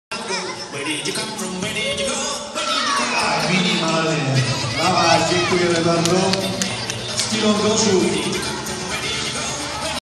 Rodzice w akcji
W ramach turnieju dla rocznika 2003 odbył sie również konkurs towarzyszący dla rodzców :-)